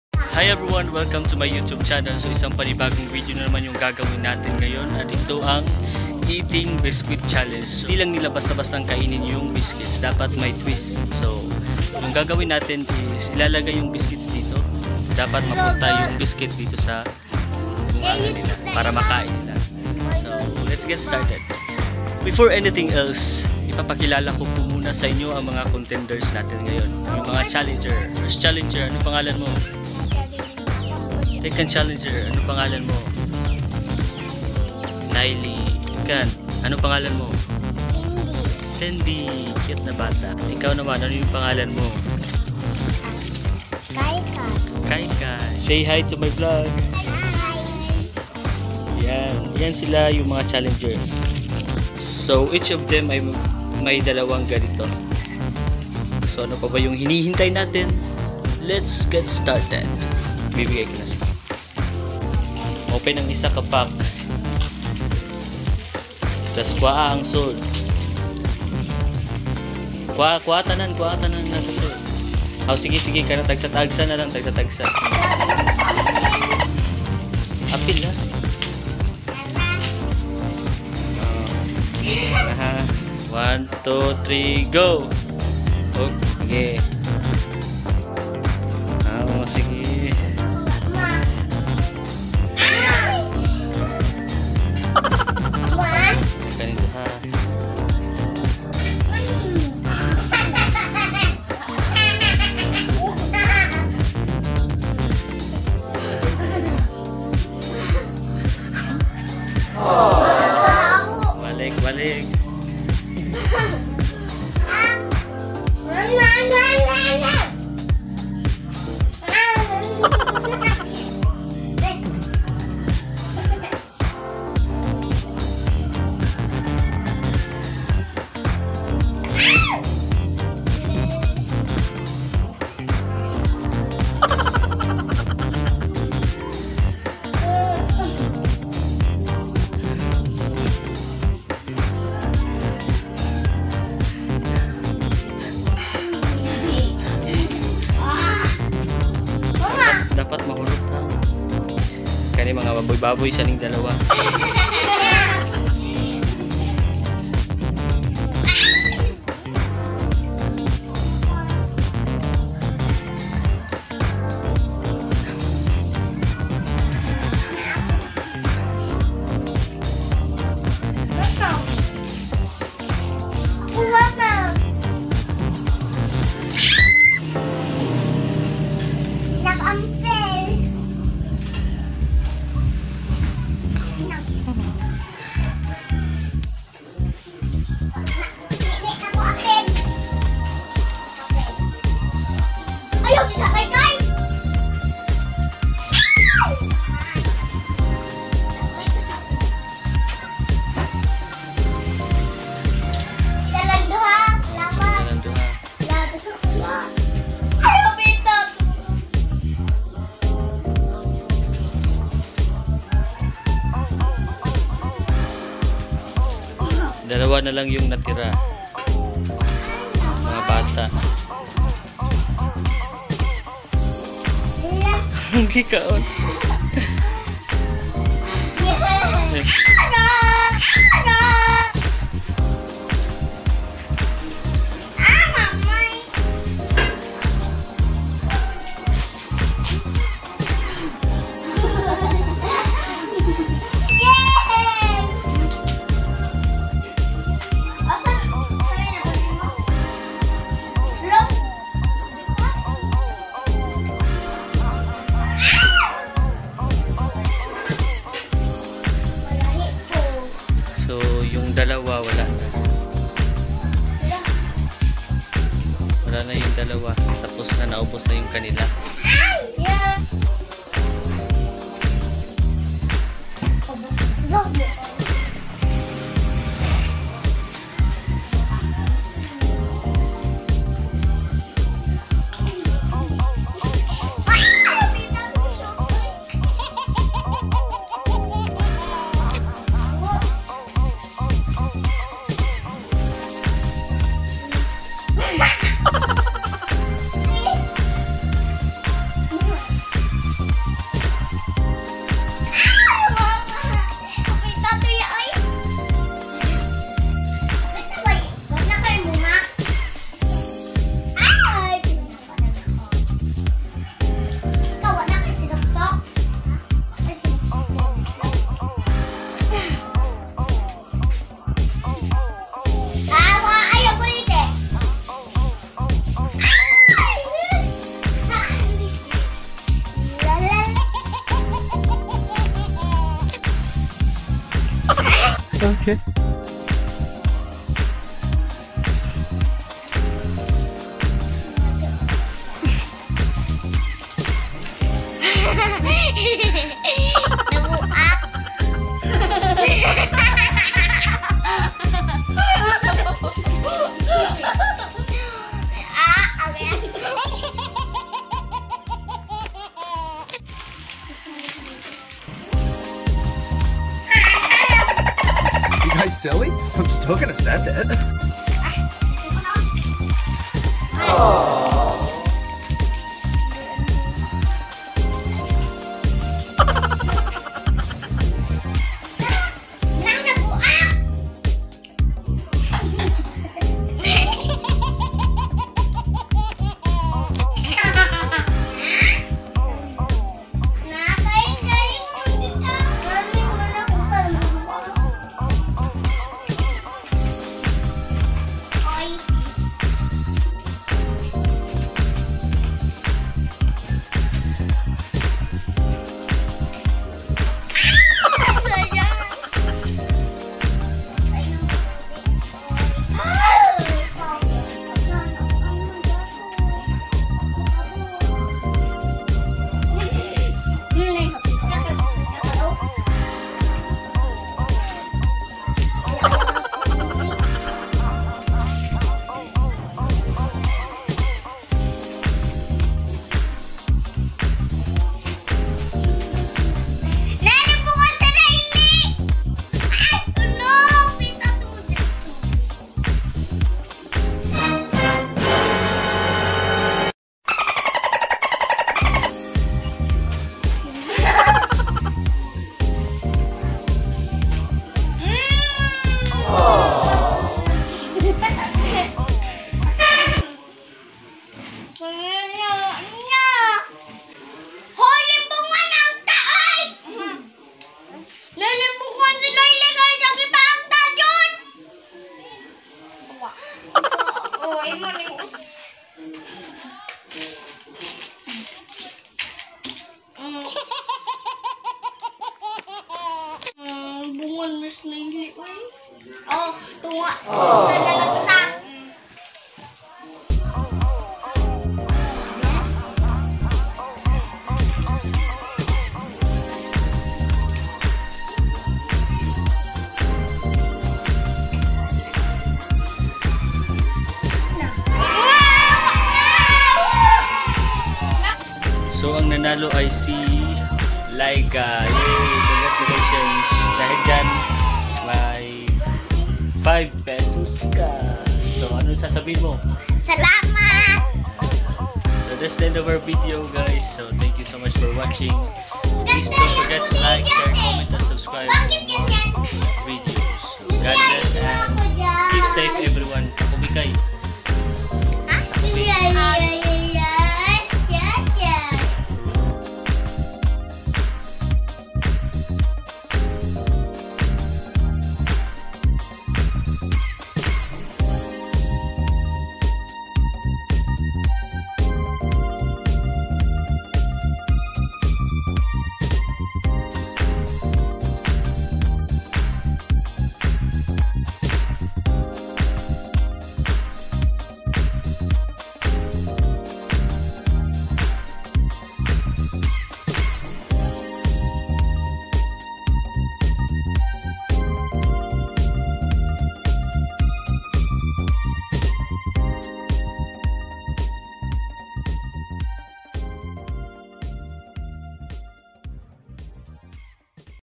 EATING BISCUIT CHALLENGE
Hi everyone!

Isang panibagong video na naman yung ginawa namin kasama yung mga kapitbahay kong mga makukulit na bata. Itong challenge na ito ay walang iba kundi ang "Eating Biscuit Challenge". I hope you will enjoy watching this video. God bless and keep safe everyone!